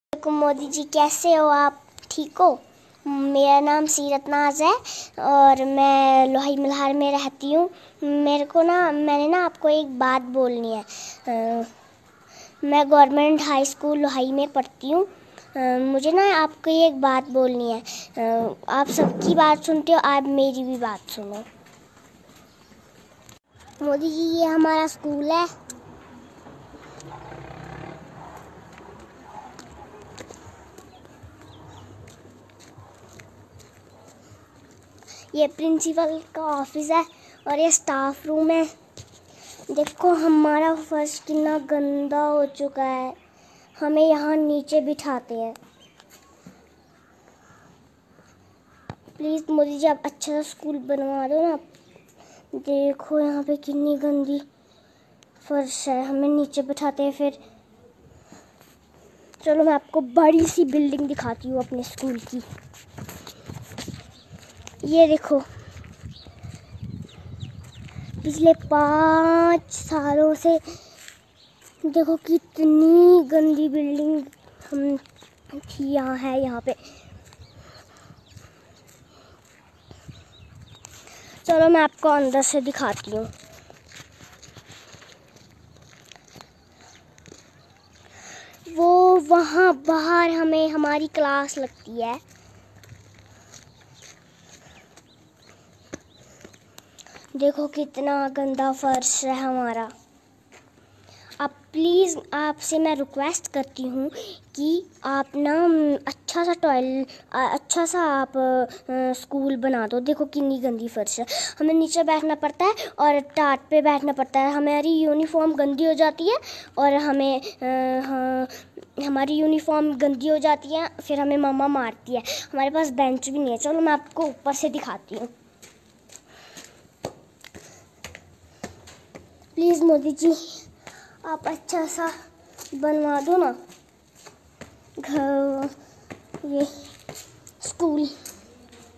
Kashmir Girl Viral Video: कश्मीरी बच्ची ने खस्ता-हाल स्कूल पर PM Modi से क्या गुज़ारिश की?
इस छोटी सी कश्मीरी बच्ची के वीडियो ने सोशल मीडिया पर वायरल हो गया है. स्कूल की खस्ता-हालत को लेकर उसने सीधे प्रधानमंत्री नरेंद्र मोदी से गुज़ारिश की. देखिये ये वायरल वीडियो.